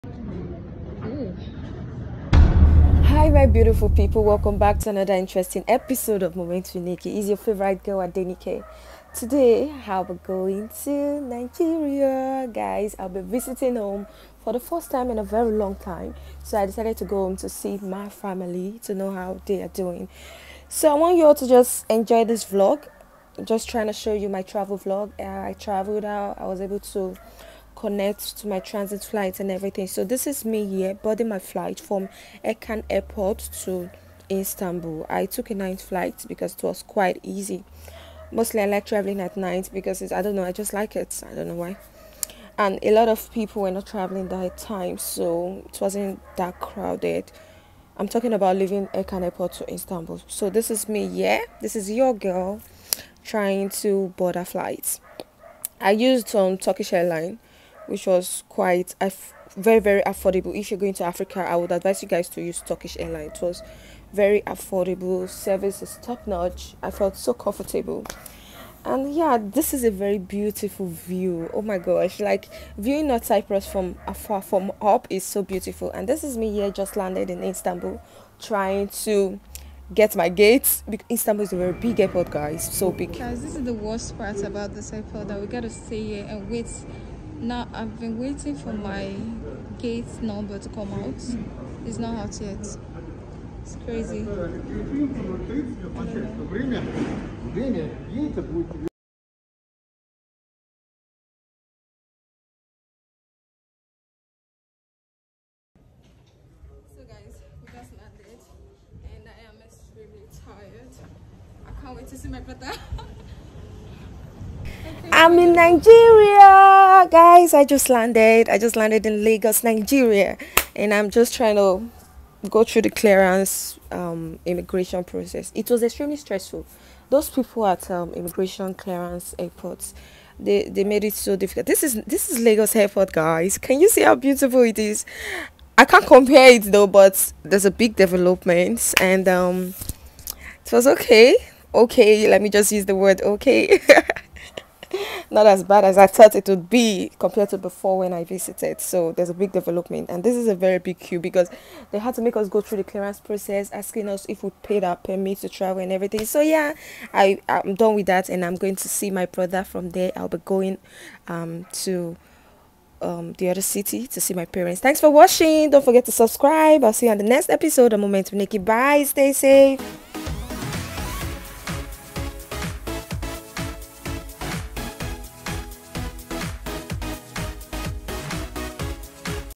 Traveling from Cyprus to Lagos with Turkish Airlines | Holiday Adventure 2021! ✈️| Join the Journey
Today’s episode is going to be a travel vlog 😊Travel with me to lagos Nigeria ..